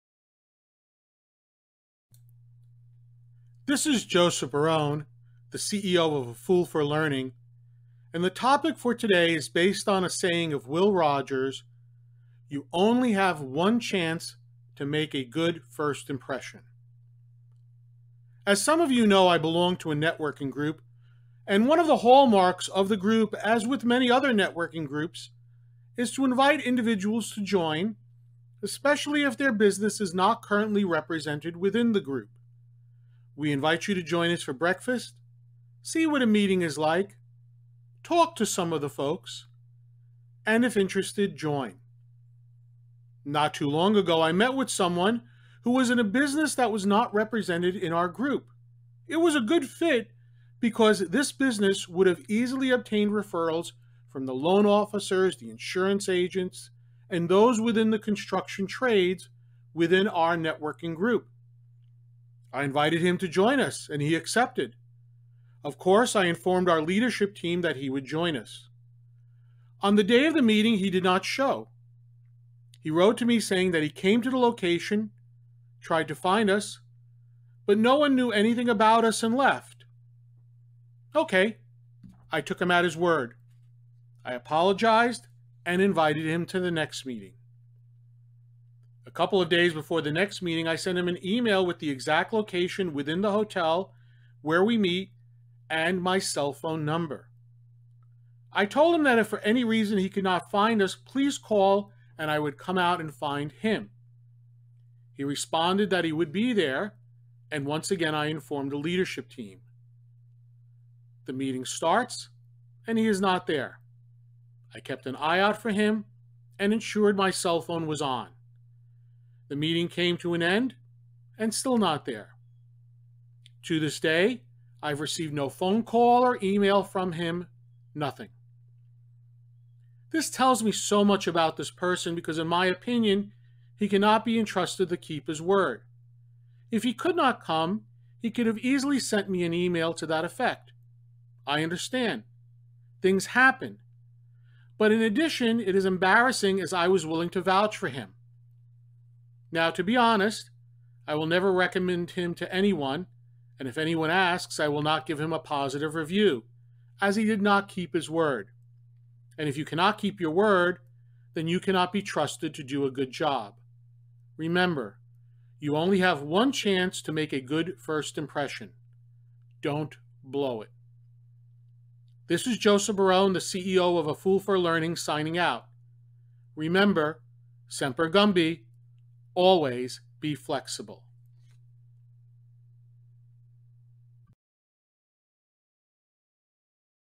Foolish Thoughts Podcast #6: You Only Have One Chance to Make a Good First Impression!
Will Rogers once said: "You only have one chance to make a good first impression."  Learn how someone blew it.